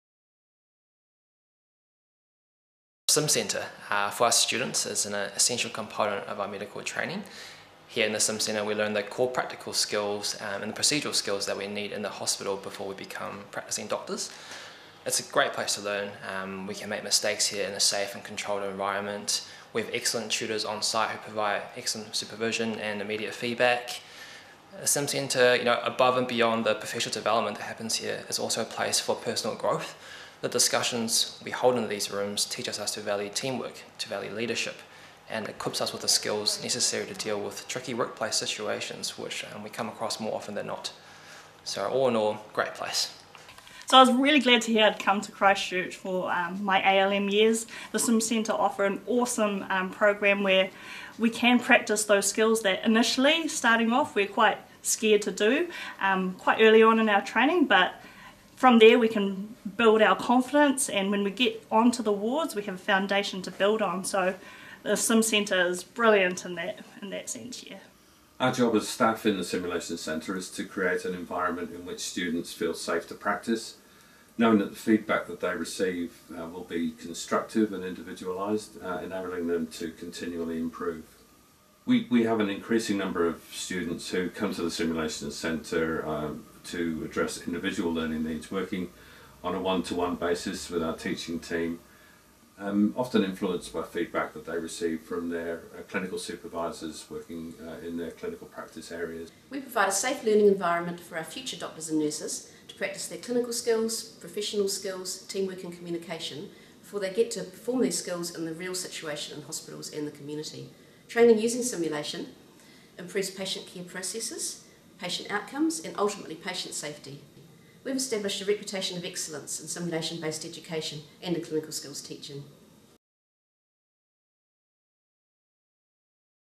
University of Otago, Christchurch Simulation Centre
The University of Otago, Christchurch Simulation Centre is a world-class facility where medical and nursing students, as well as working health professionals, do simulation-based training in a broad range of clinical and professional skills. It is a key part of Otago's undergraduate medical programme and allows students the opportunity to practice situations they will encounter in the real world in a safe environment.